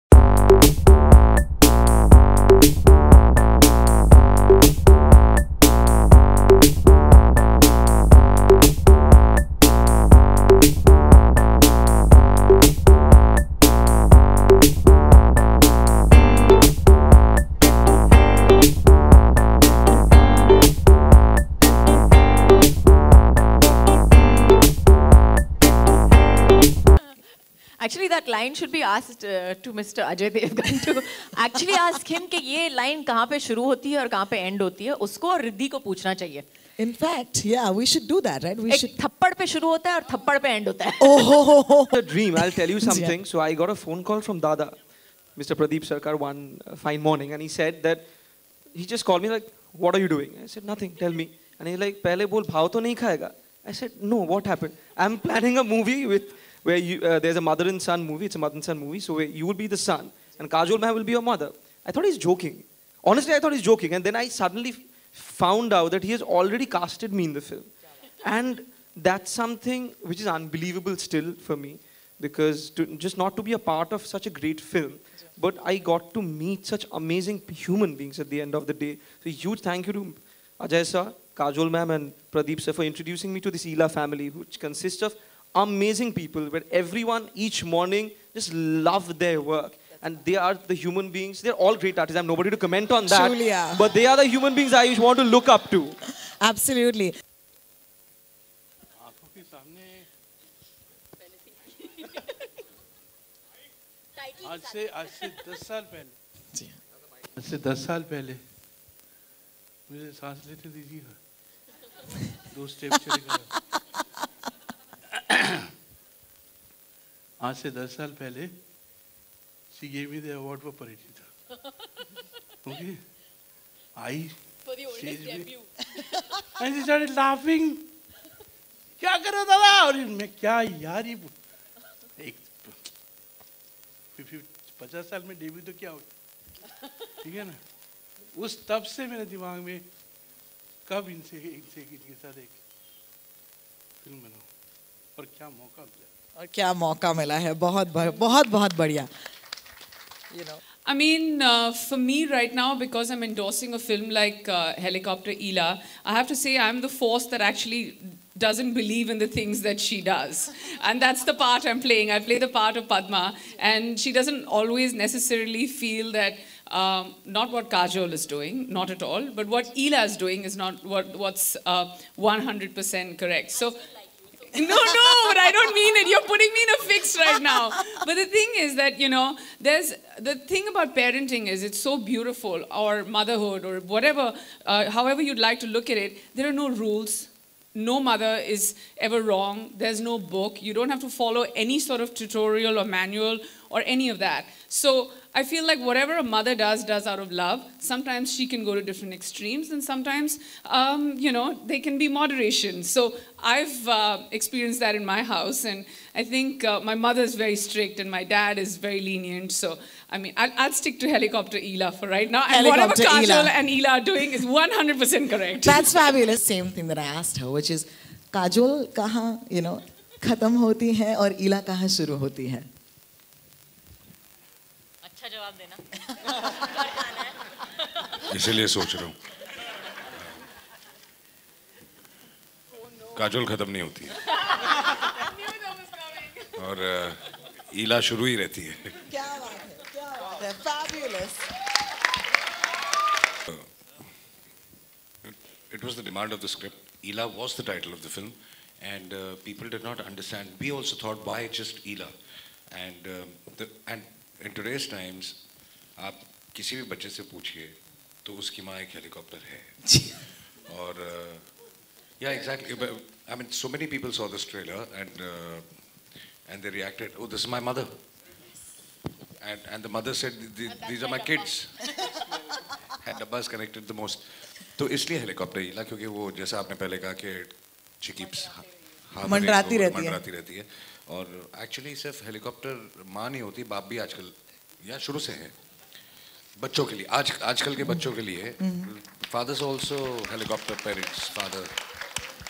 Helicopter Eela Trailer Launch | Kajol | Bollywood Celebrities Interview 2018
Kajol starrer Helicopter Eela trailer is out. Kajol plays the role of a free-spirited and cool single mother, who doesn’t even think twice before taking admission in her son’s college. Riddhi Sen is seen playing Kajol’s son in the movie, directed by Pradeep Sarkar.